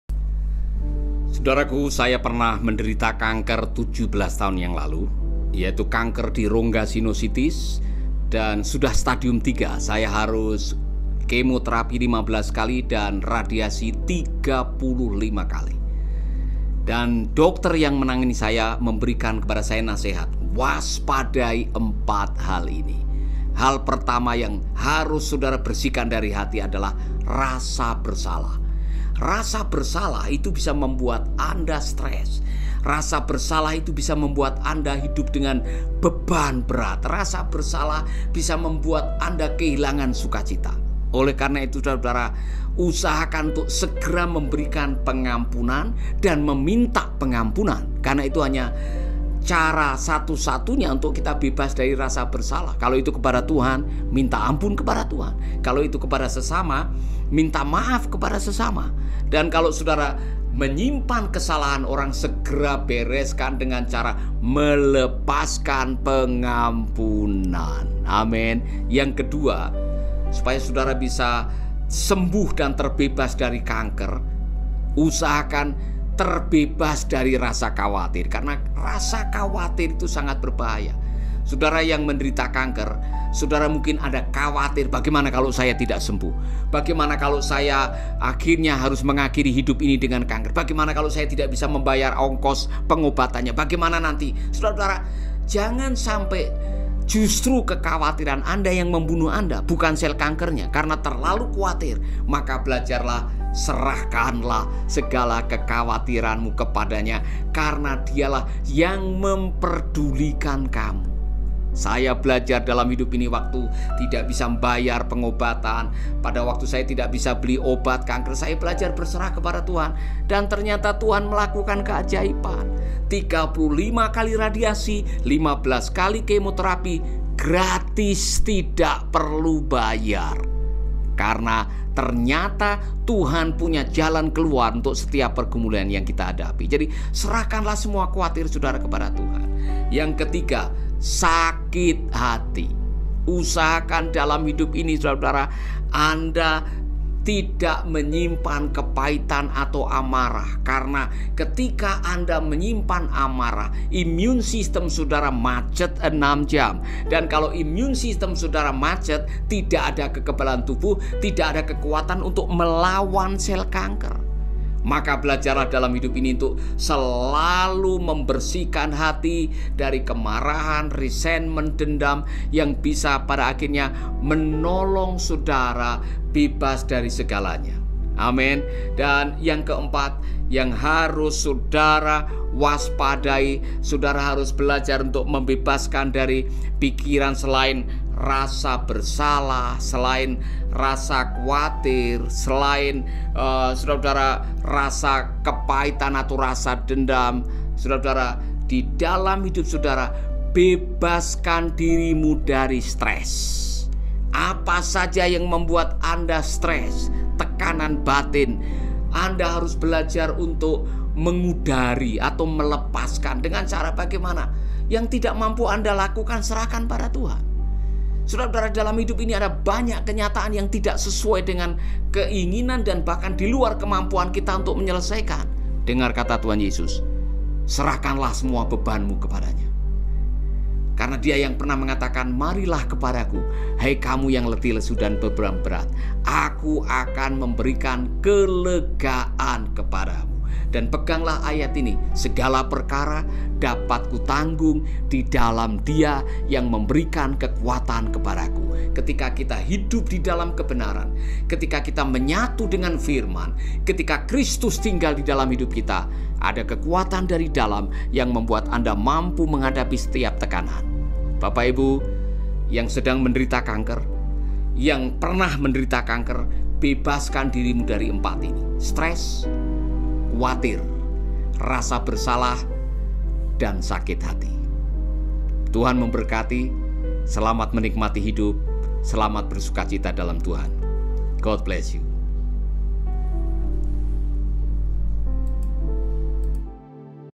Nasehat Buat Penderita Kanker
Empat hal ini harus dikeluarkan dari diri anda. Kepahiyan, amatah, rasa bersalah dan kuatir. Jaga harapan hidup dan buatlah dirimu bersukaciya.